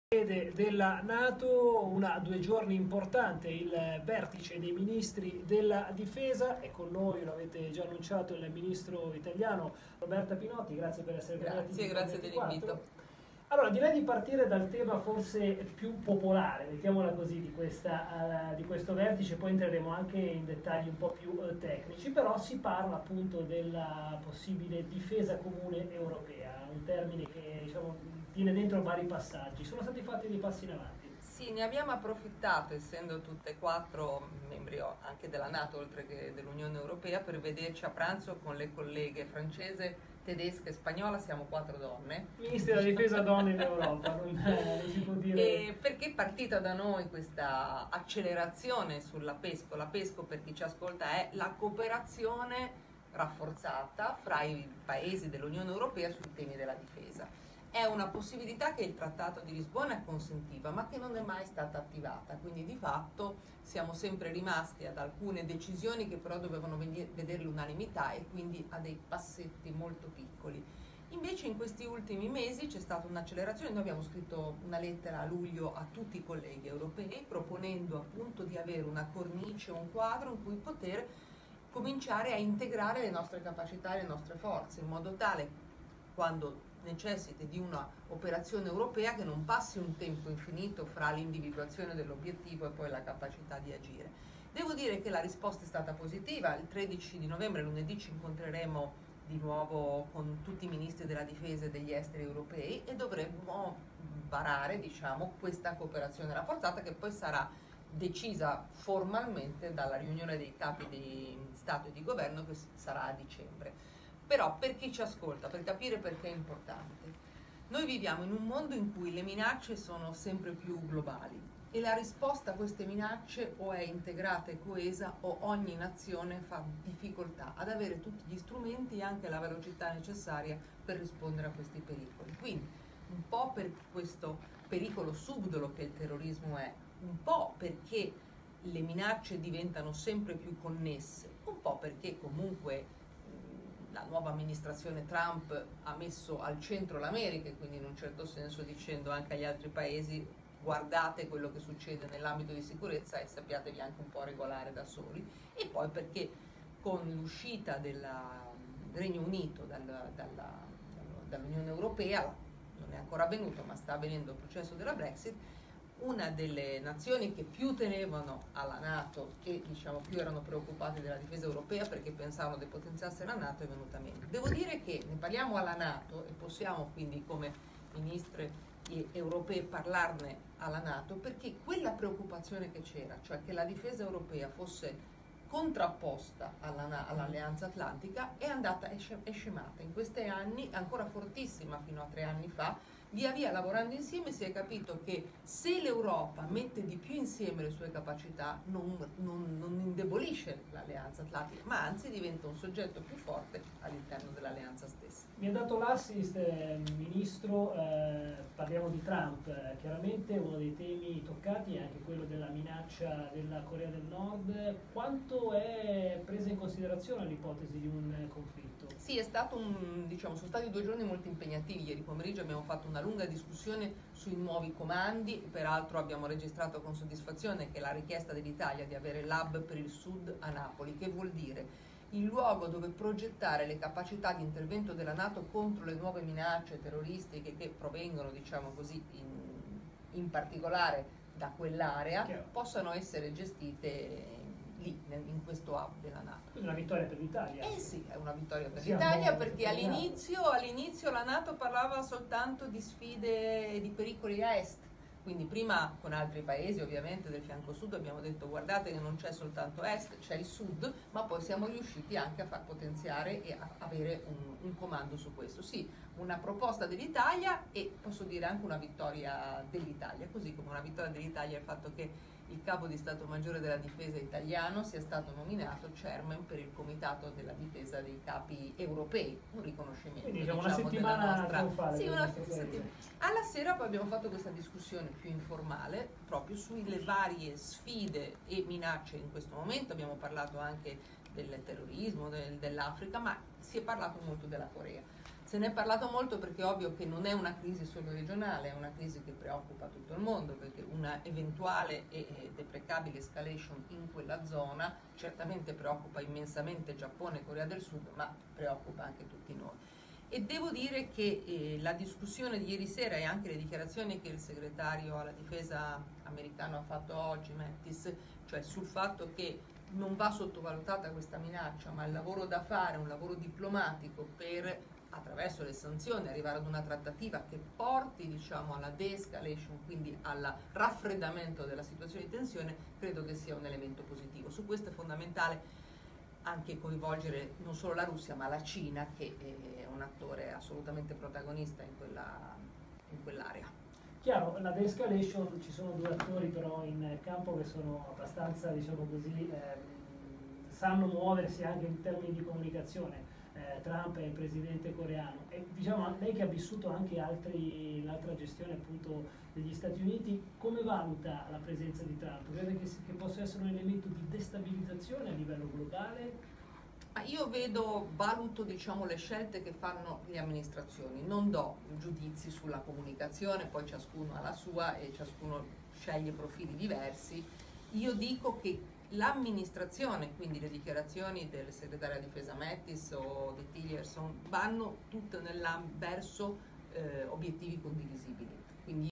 Ministeriale NATO - Intervista al Ministro della Difesa Roberta Pinotti, 9 novembre 2017
Intervista di Leonardo Panetta (Mediaset) al Ministro della Difesa, Sen. Roberta Pinotti, a margine dei lavori della Ministeriale NATO, 9 novembre 2017